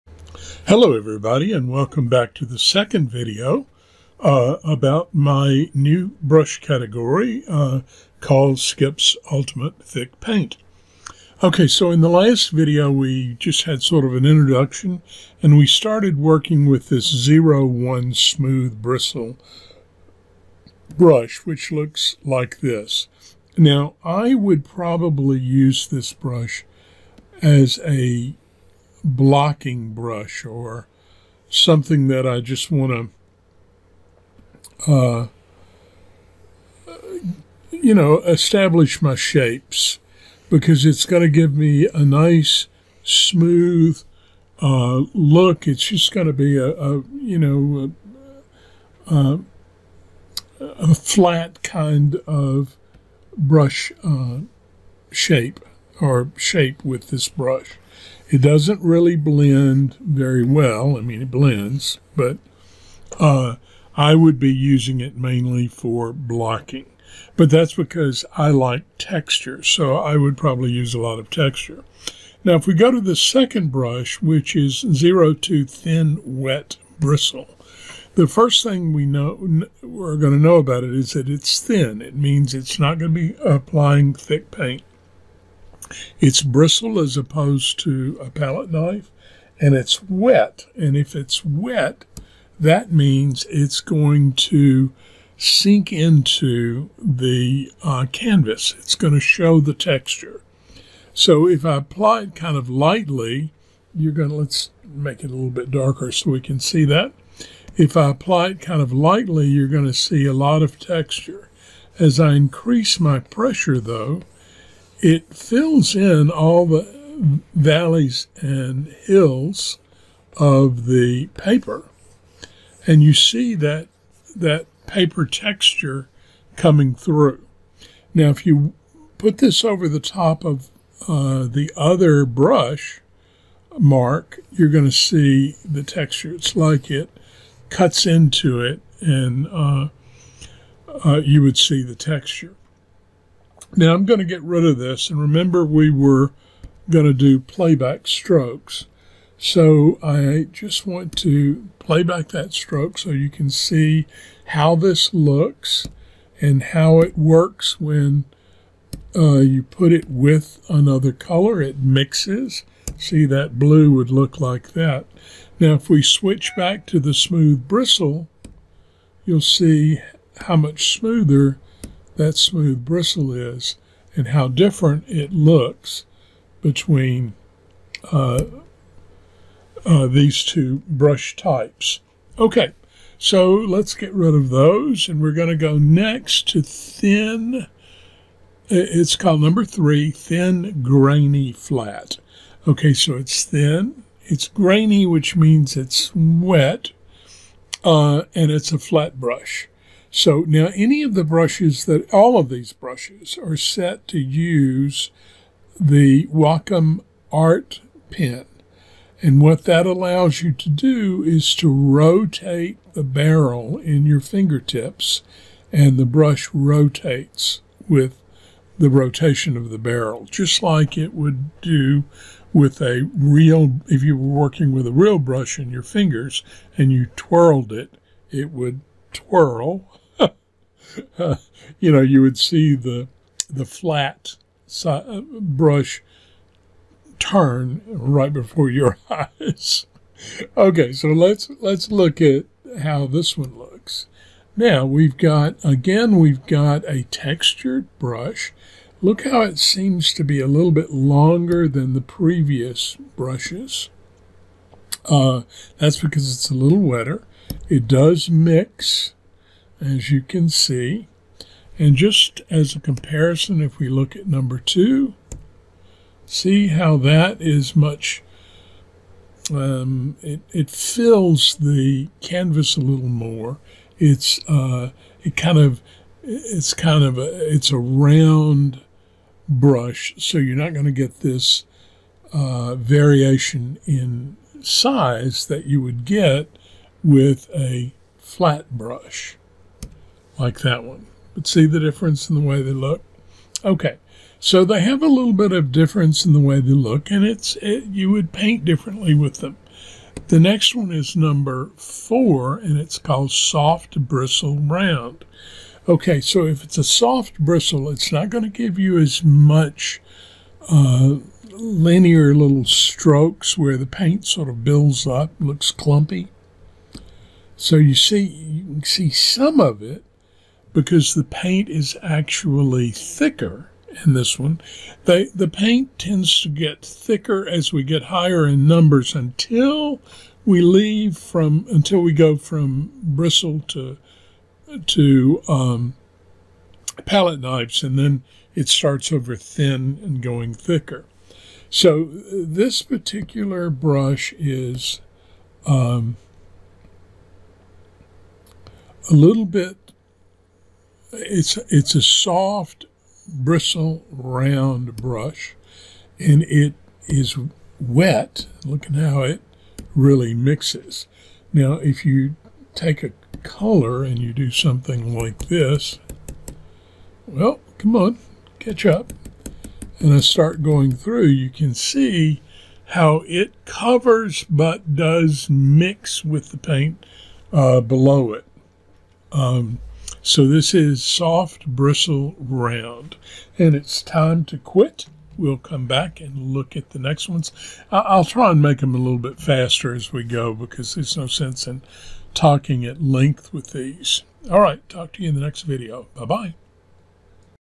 002-Ultimate Thick Paint - First four
Video 002.  A series about Skip's Ultimate Thick Paint category for Corel Painter 2020 software.  The brushes are available on my blog.